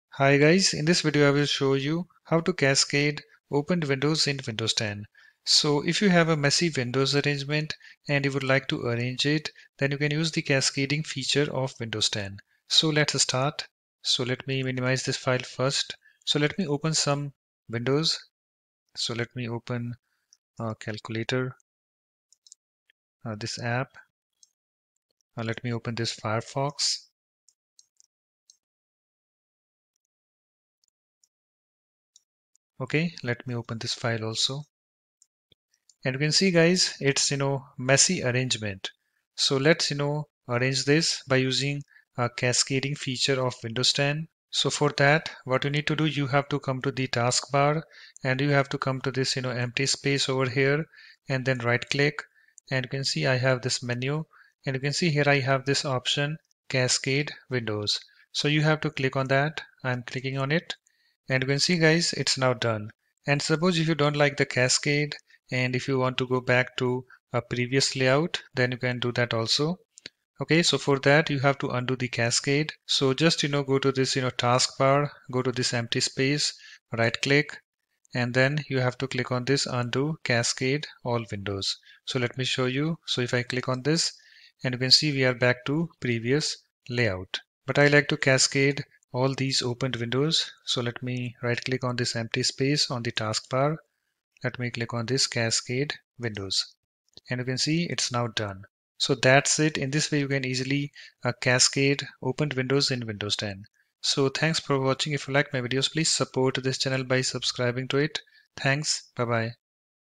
How to Cascade Opened Windows in Windows 10?
In this Video, I will show you 'How to Cascade Opened Windows in Windows 10.'